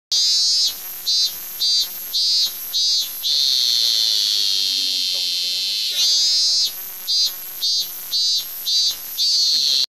my japanese lullaby
This is the ambient sound that will be there for you while you're relaxing in a park or even going to bed at home.